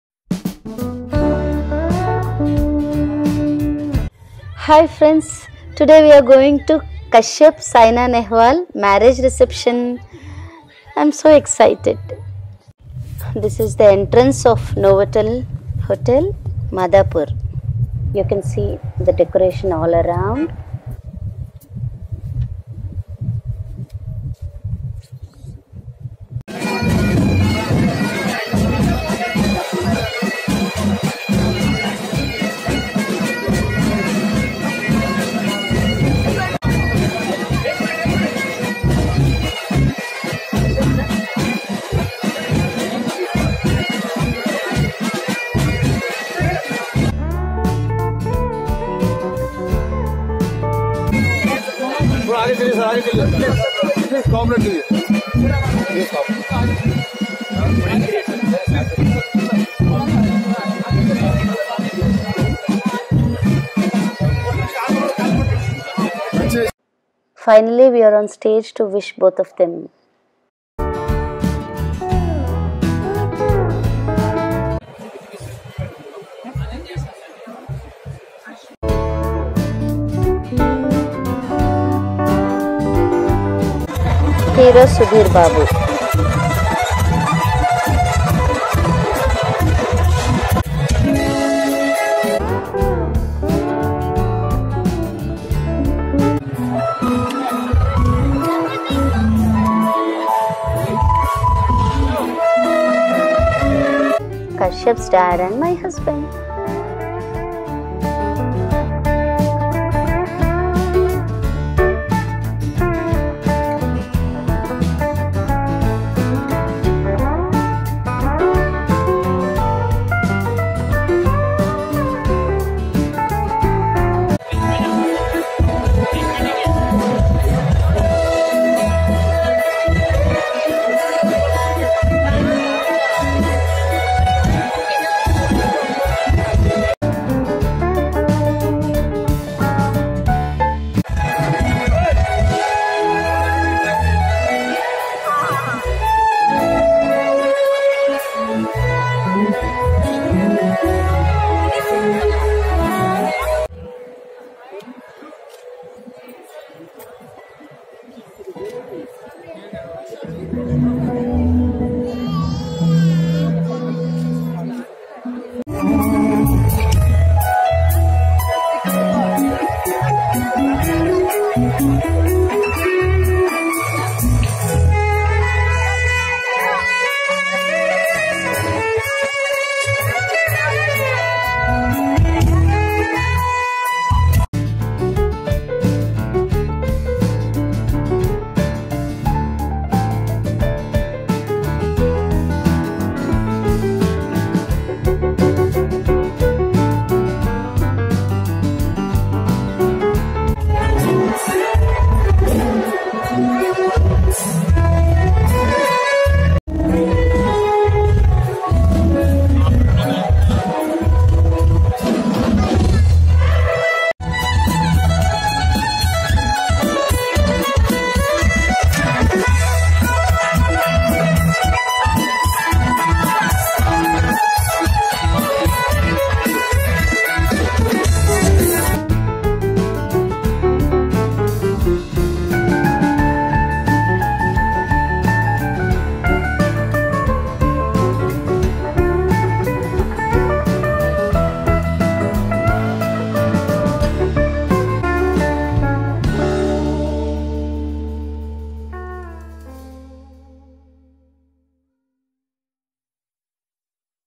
Saina Kashyap Wedding Reception Full Vlog
Minister Kavita Garu, Film Stars, Athletes And Many Important Guests have Blessed The Cute Couple.